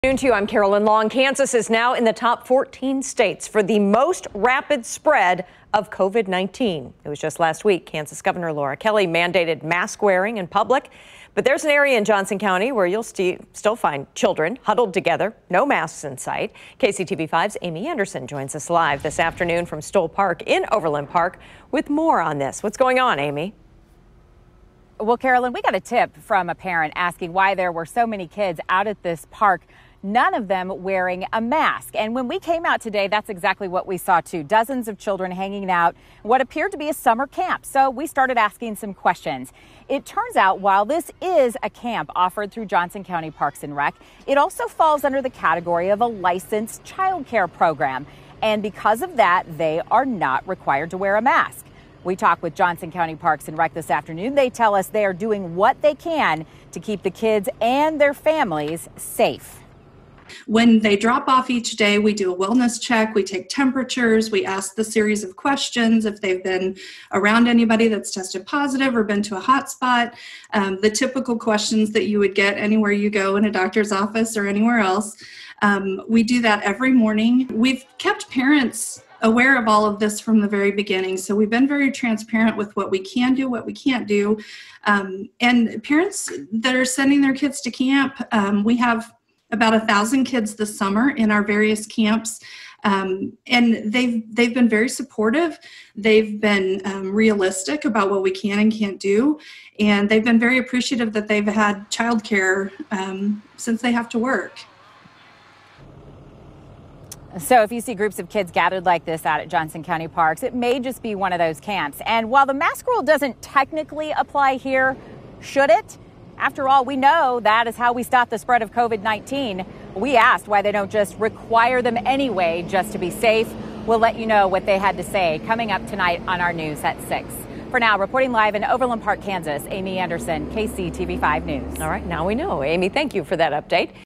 Concerned parent questions why kids attending Johnson County Parks and Rec camp are unmasked
Kansas has landed itself on a not so flattering list, as of Tuesday the state is in the top 14 states for the most rapid spread of the coronavirus and the least restrictions on activities.